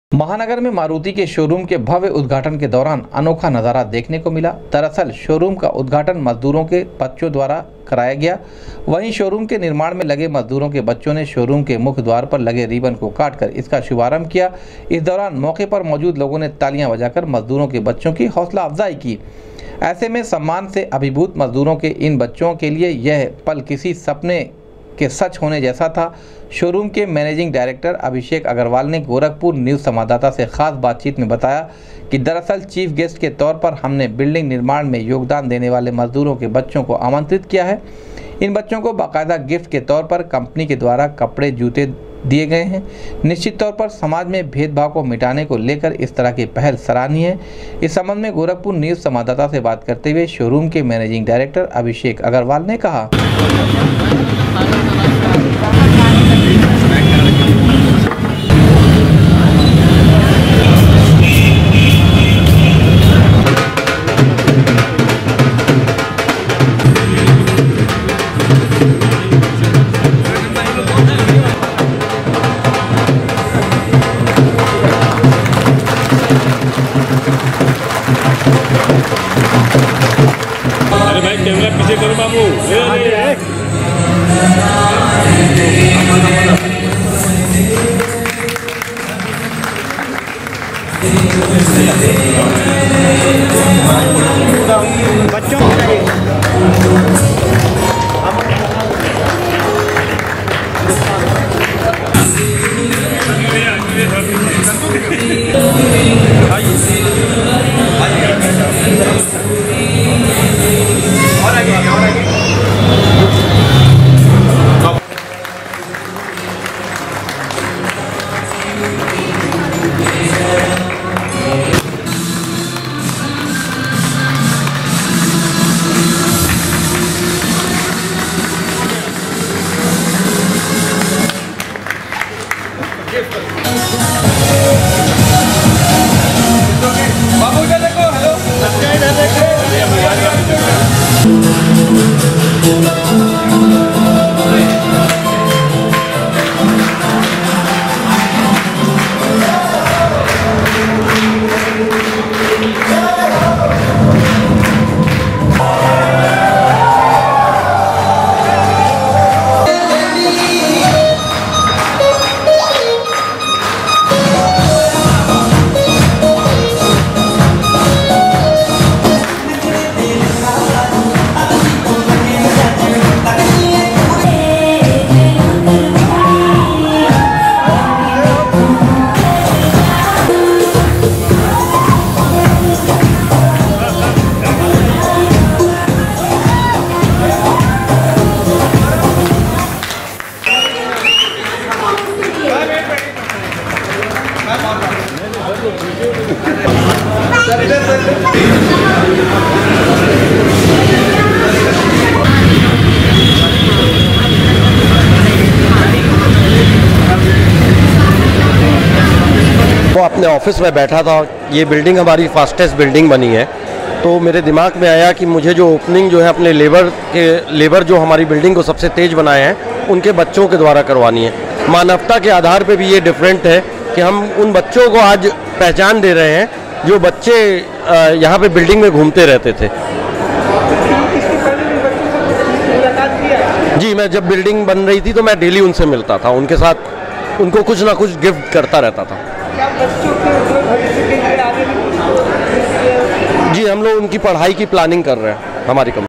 मारुति के नये शोरूम का भव्य उद्घाटन के दौरान देखने को मिला अनोखा नजारा, शोरूम
मारुति के नये शोरूम का भव्य उद्घाटन के दौरान देखने को मिला अनोखा नजारा, शोरूम के निर्माण में लगे मजदूरों के बच्चों ने, शोरूम के मुख्य द्वार पर लगे रिबन को काटकर किया शुभारंभ।